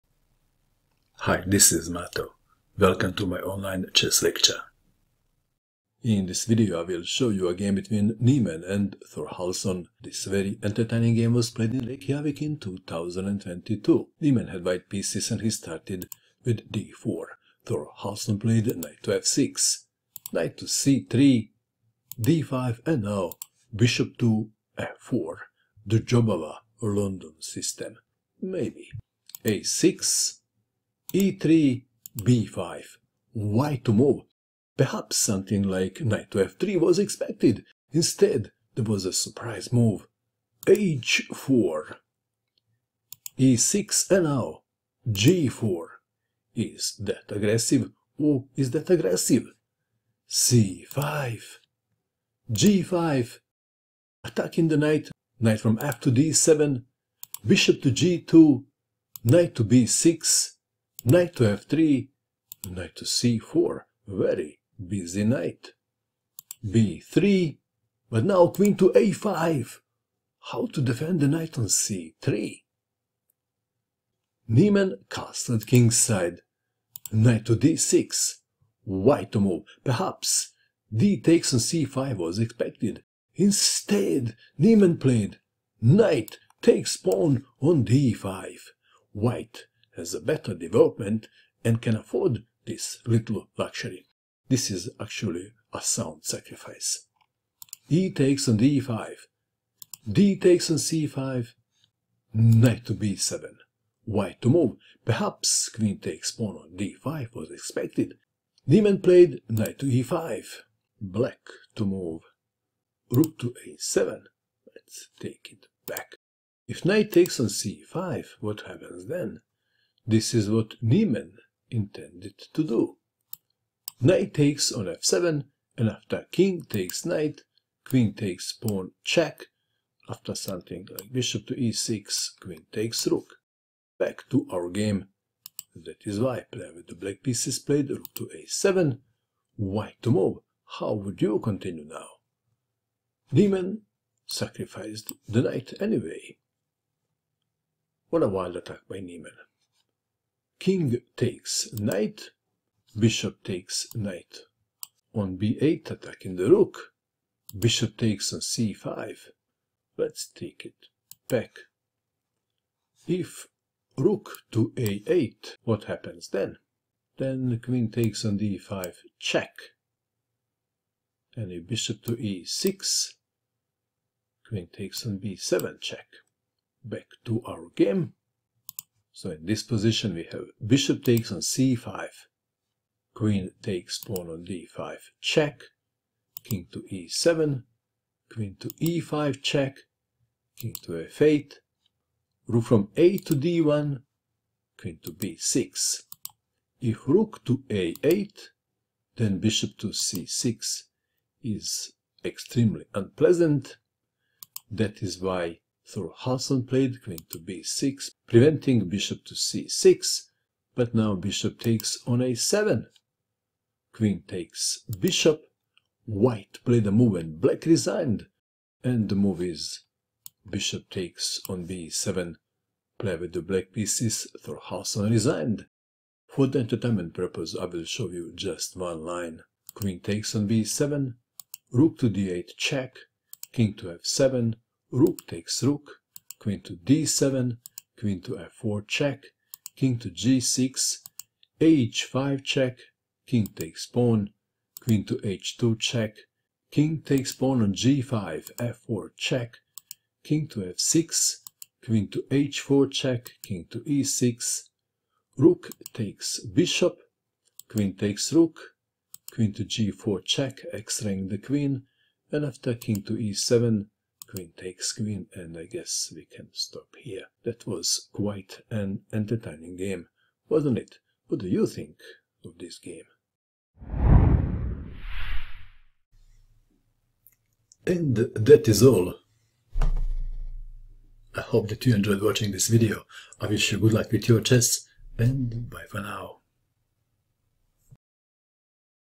Why Is Everybody Talking About Niemann? | Niemann vs Thorhallsson: Reykjavik Open 2022
Hans Moke Niemann vs Throstur Thorhallsson
Kvika Reykjavik Open 2022, Reykjavik ISL, rd 9, Apr-12
Indian Game: General (A45)  ·  1-0
1. d4 Nf6 2. Nc3 d5 3. Bf4 a6 4. e3 b5 5. h4 e6 6. g4 c5 7. g5 Nfd7 8. Bg2 Nb6 9. Nf3 Nc4 10. b3 Qa5 11. O-O Nd6 12. Nxd5 exd5 13. dxc5 Nb7 14. Ne5 Ra7 15. Nxf7 Kxf7 16. Bxb8 Bxc5 17. Qxd5+ Ke7 18. Qe5+ Kf8 19. Rad1 Qb6 20. Bxa7 Qxa7 21. Bxb7 1-0